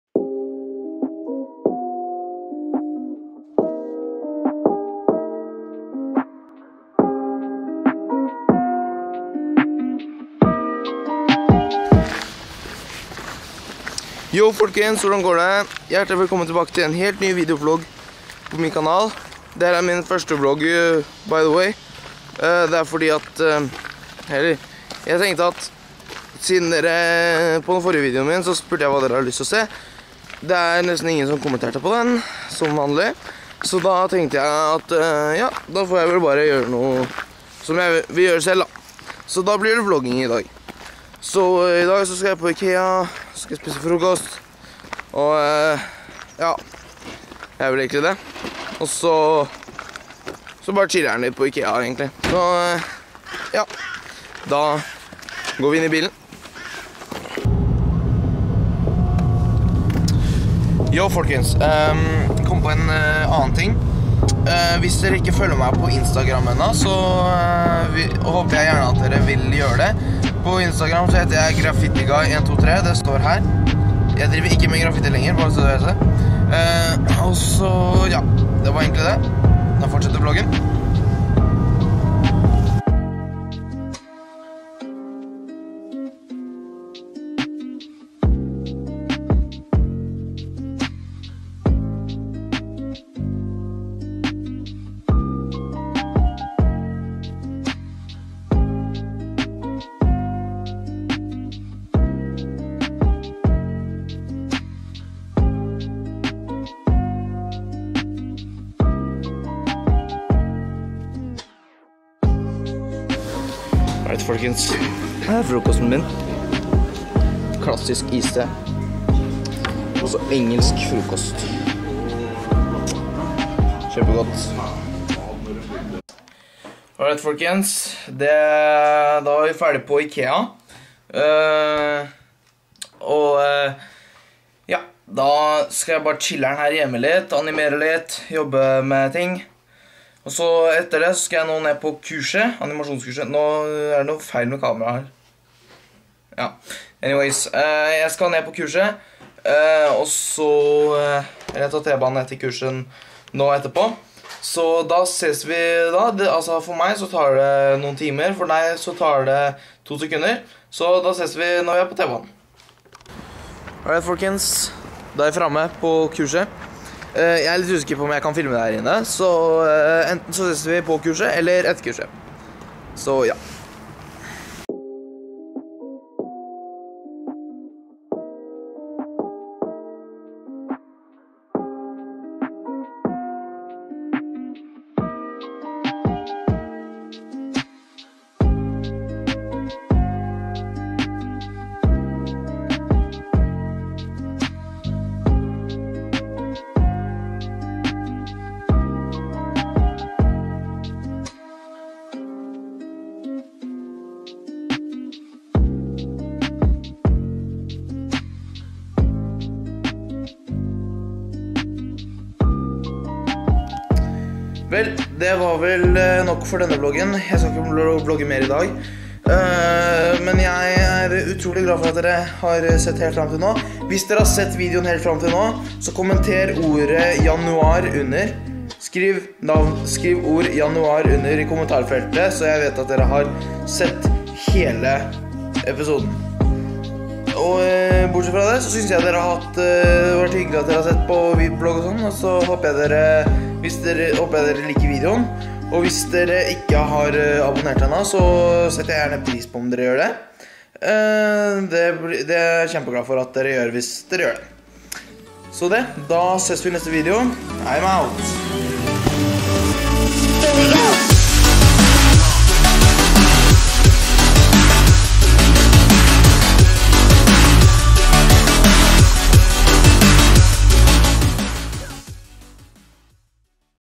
Vlog 22.01.2020
hei og velkommen til min første vlog!

i dag drar jeg på ikea og animasjonskurs

hvis du likte videoen håper jeg at du trykker på liker